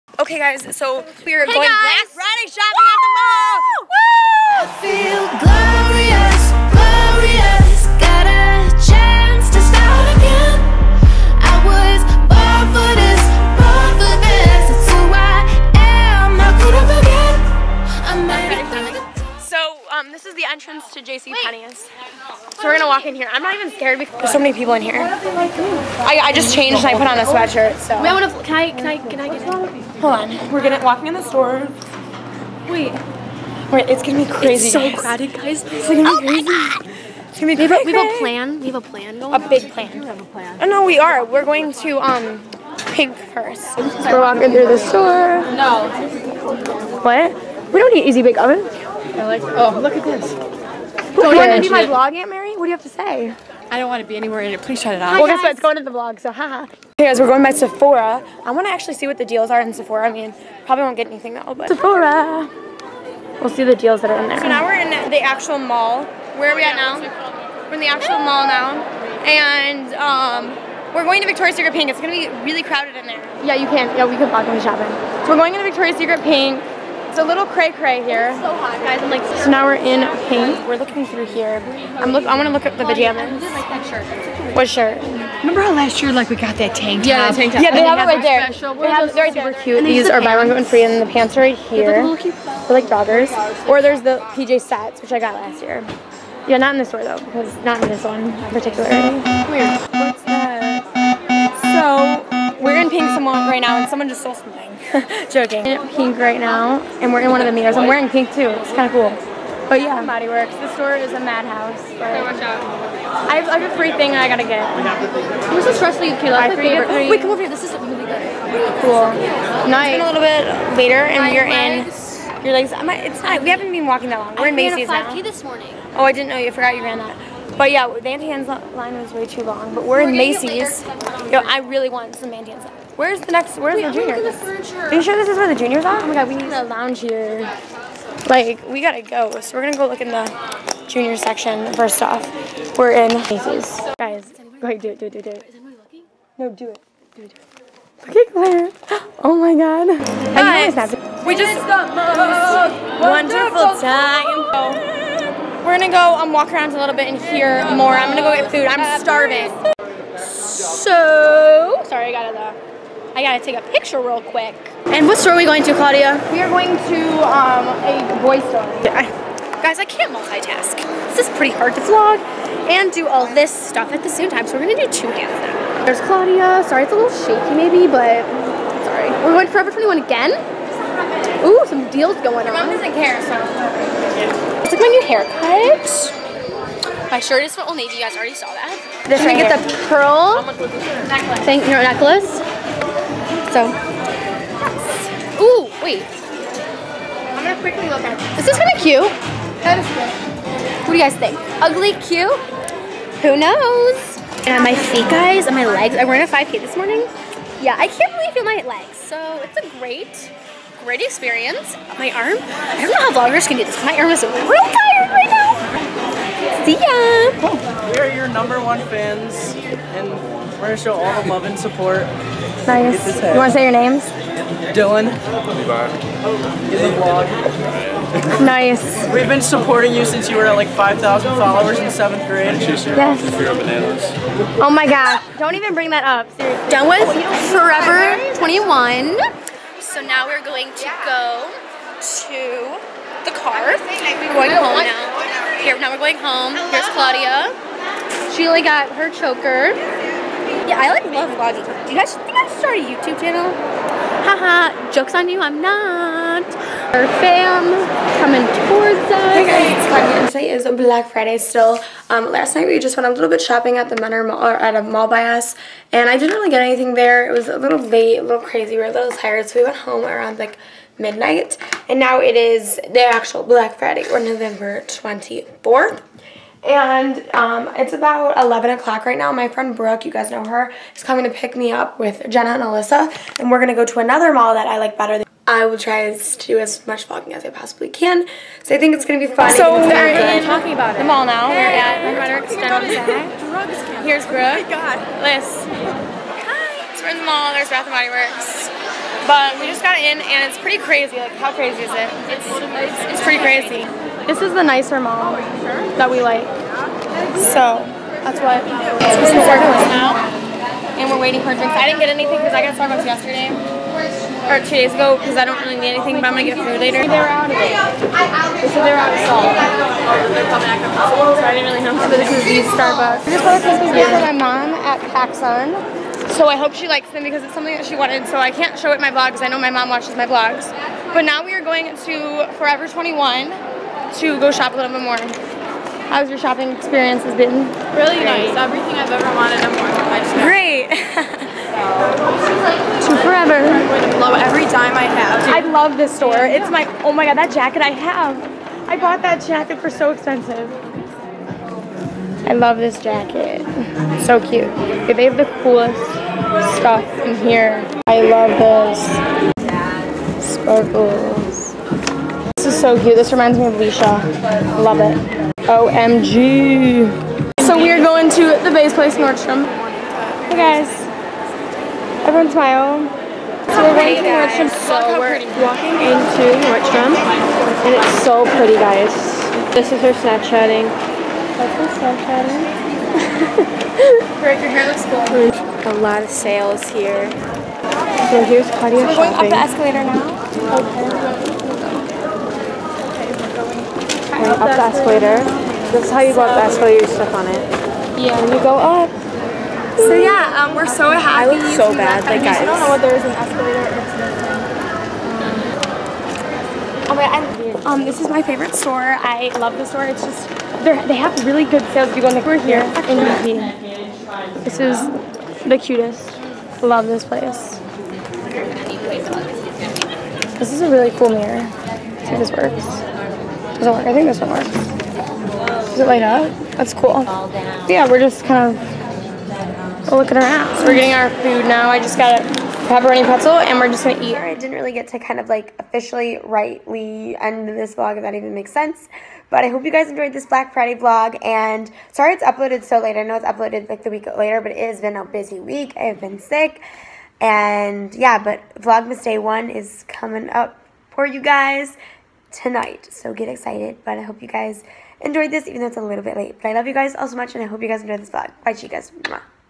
Black Friday Shopping Maddness
Black Friday shopping with my cousin at night and with my friends in morning. Shopping was crazy but also a ton of fun! I hope you all enjoyed it even tho its up a week late;(. Love u guys all so much and hope everyone is have a amazing day. Love u chicas!